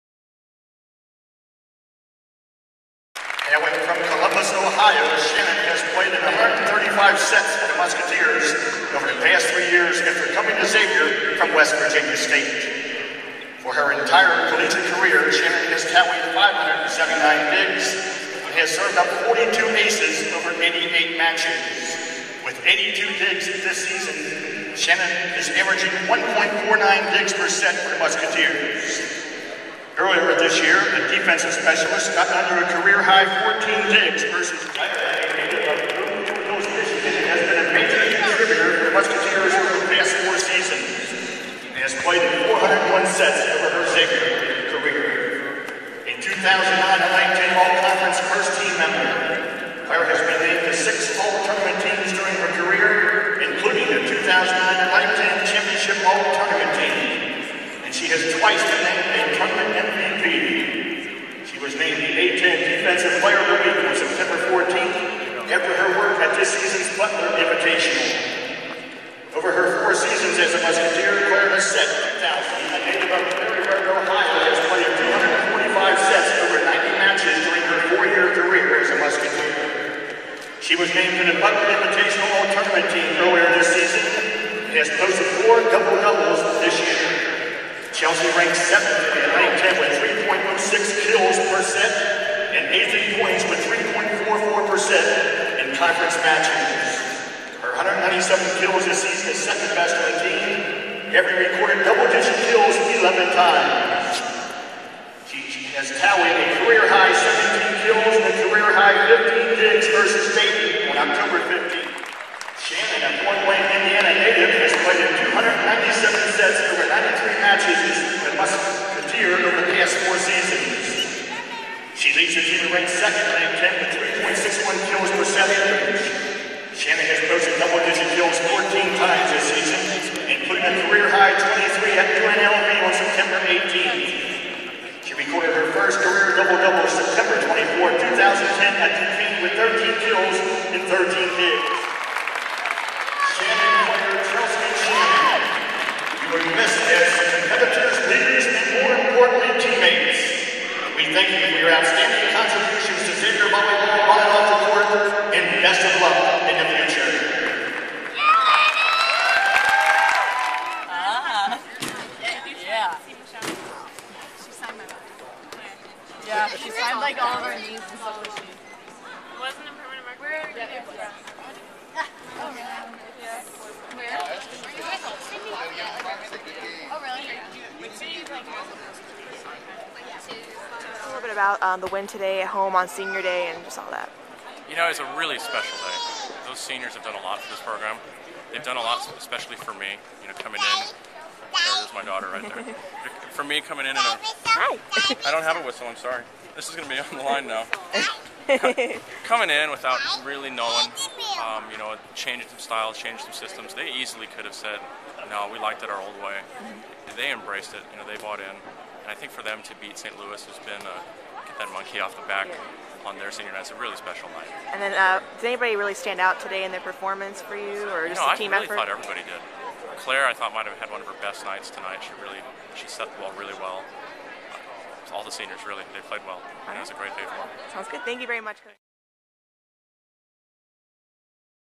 Xavier Volleyball Senior Day vs. Saint Louis Oct. 31, 2010
Xavier Volleyball Senior Day vs. Saint Louis Oct. 31, 2010. Four senior team members were recognized prior to the match against Saint Louis. The Musketeers defeated the Billikens, 3-0